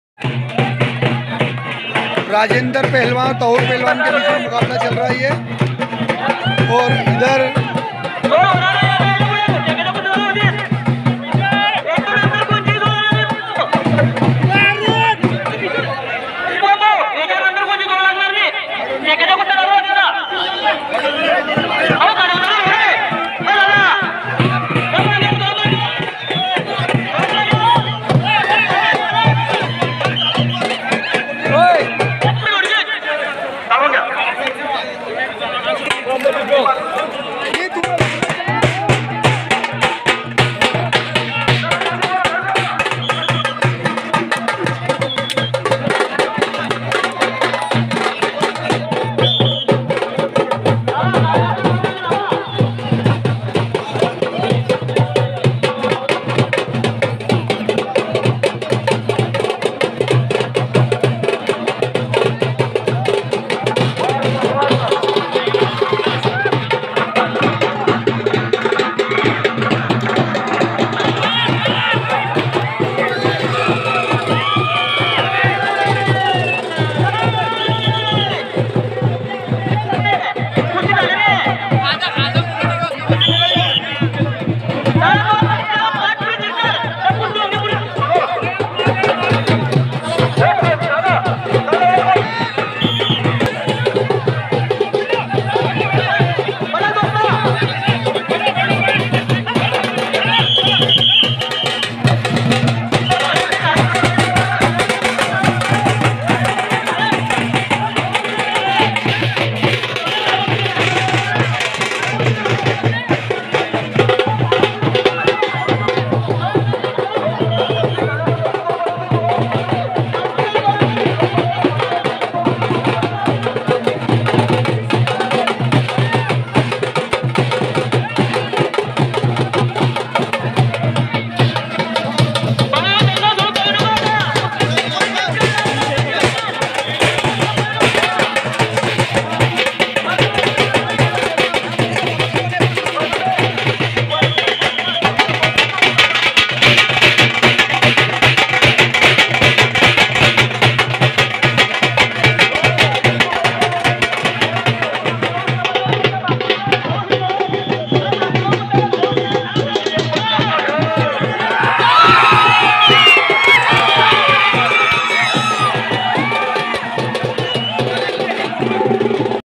तहूर पहलवान मालेगाव विरुद्ध राजेंद्र पहलवान लोहारा{maharashtra kushti dangal}
स्वर्गीय सुनील बोरसे पहलवान की याद में कुश्तीयों का विराट आम दंगल आयोजक दोस्ती यारी ग्रुप जामनेर पुरा जामनेर महाराष्ट्र{8/1/2023}